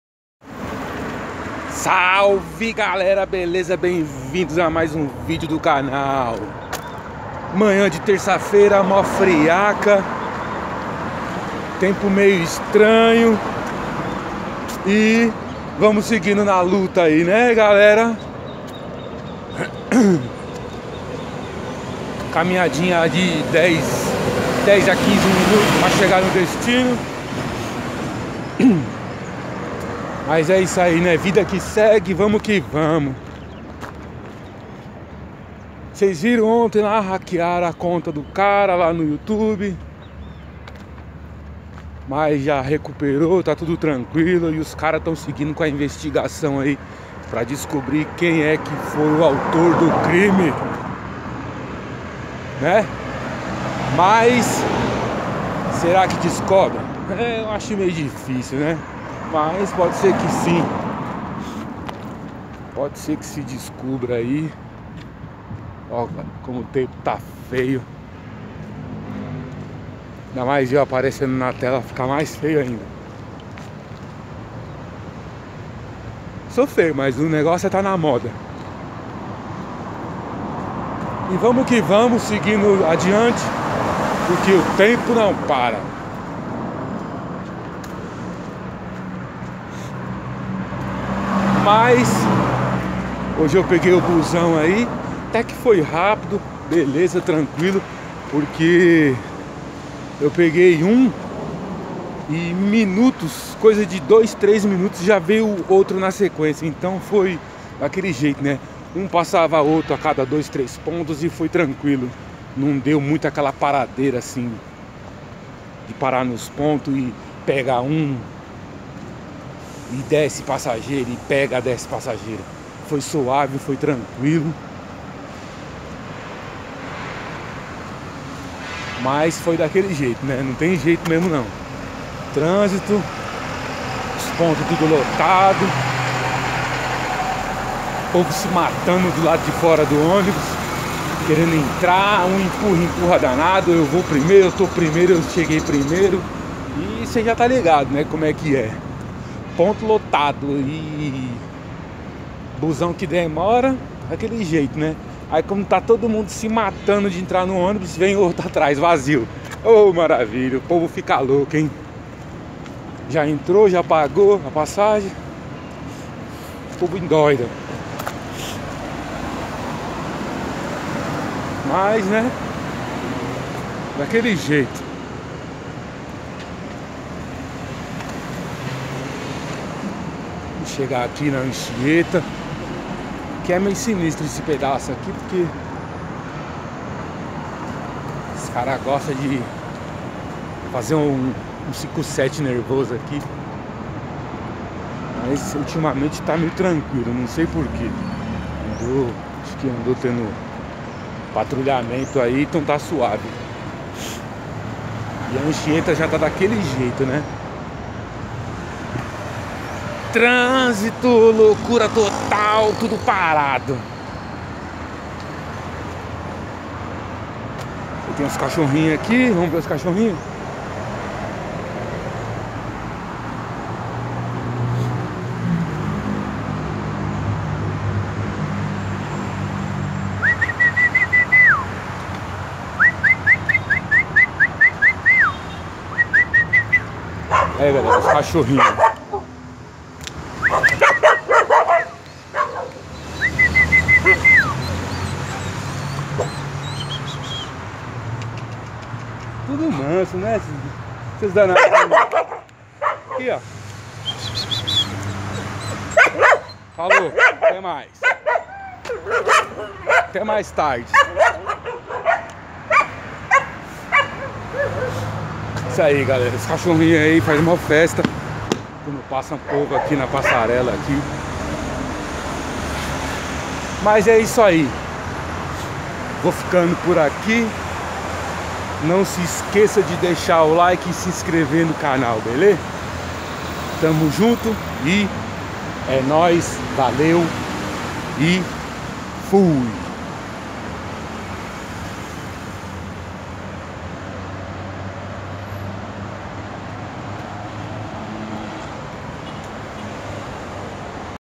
OLHA A FELICIDADE DOS CACHORROS QUANDO PASSA ALGUEM ‹ BUG STOFEL ›
CAMINHADA TODOS OS DIA OLHA A FELICIDADES DOS CACHORROS